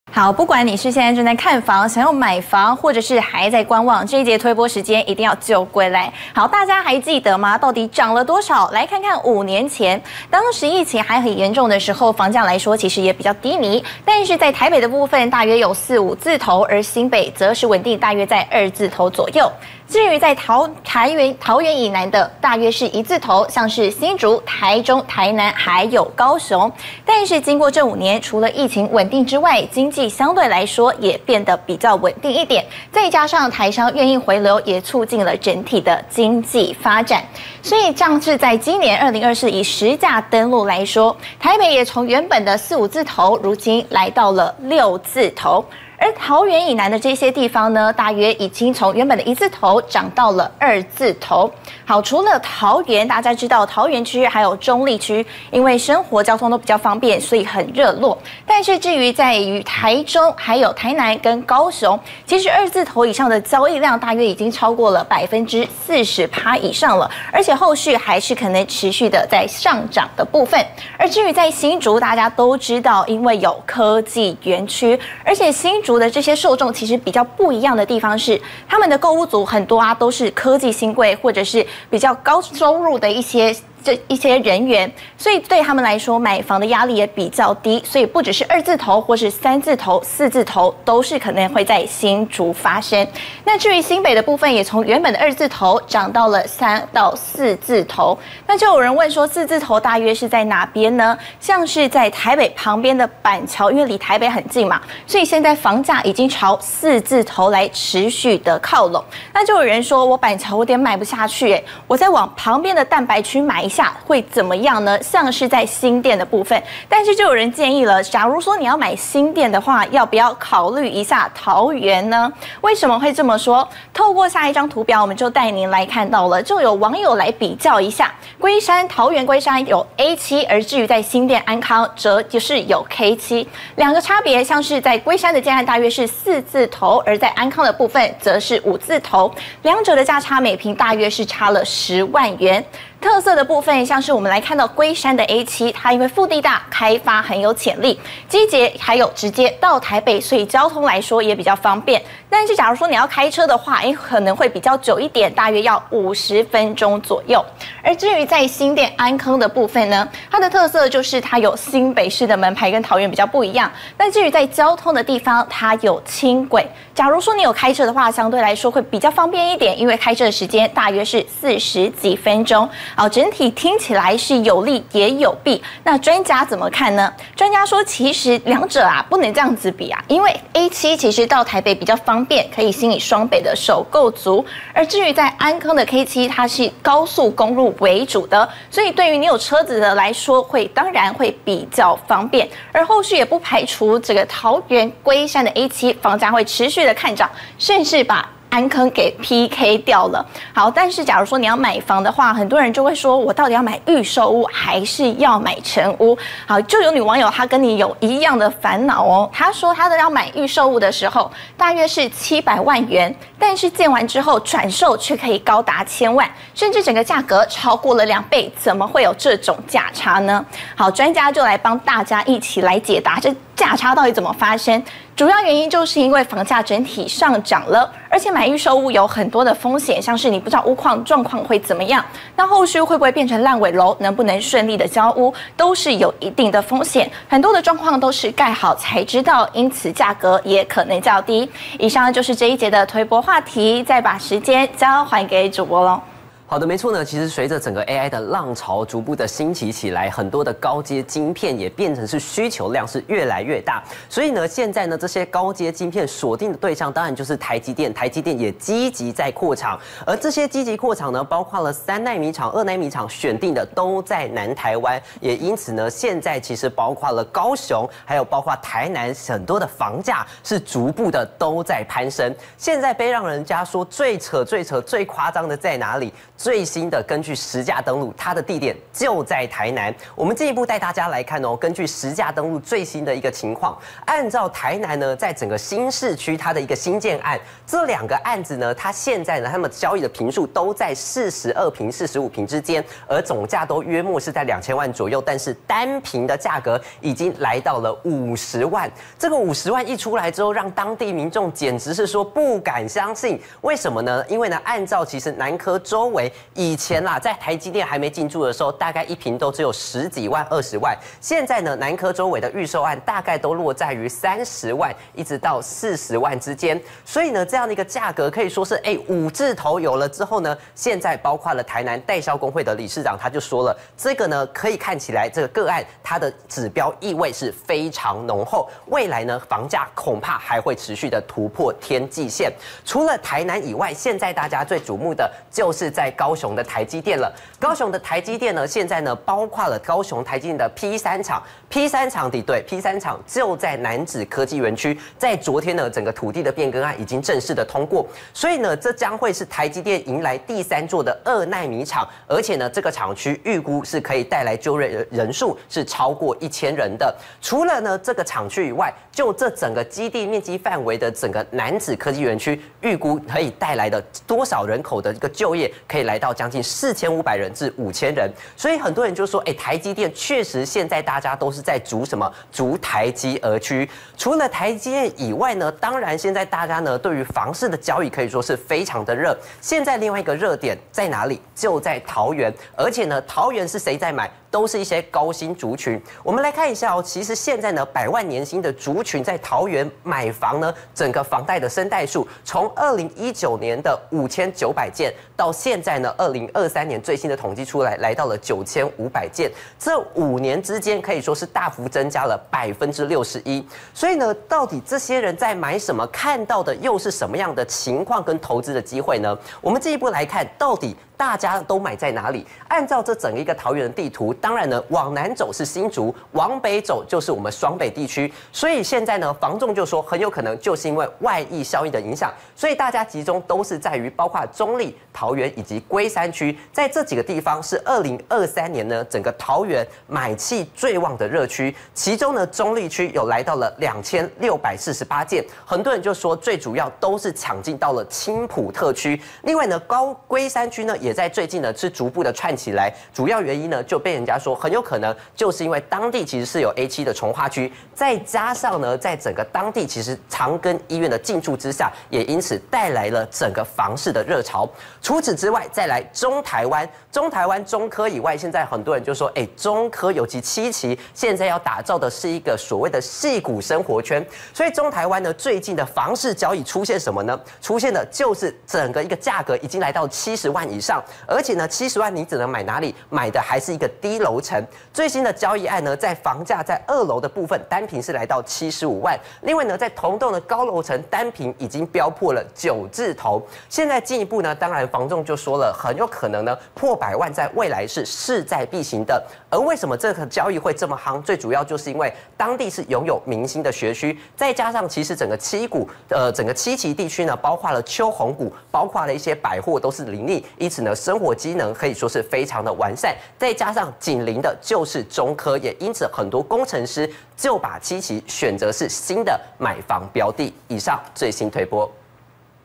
【地產王日報】房價板塊移動！桃園以南五都漲至2字頭/逐台積電而居！ 台南預售房價創高 單坪首飆5字頭
【絜安推播】房價板塊移動！桃園以南五都漲至2字頭
【蔡明叡推播】最新》逐台積電而居！ 台南預售房價創高 單坪首飆5字頭